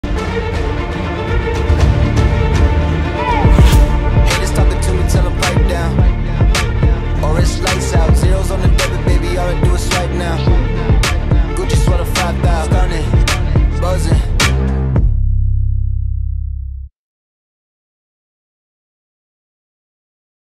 Marvel Strada Insulated Stainless Steel Shaker Bottles by BlenderBottle
Our new officially licensed Marvel designs are here to take on your toughest workouts. Etched on our Strada Stainless Steel shaker—you'll get premium features like double-wall insulated stainless steel, rounded base, lockable lid, and a push-button open flip cap. Get your favorite Marvel heroes including Captain America, Iron Man, Spider-Man, Captain Marvel, Black Panther, Black Widow, and Thor.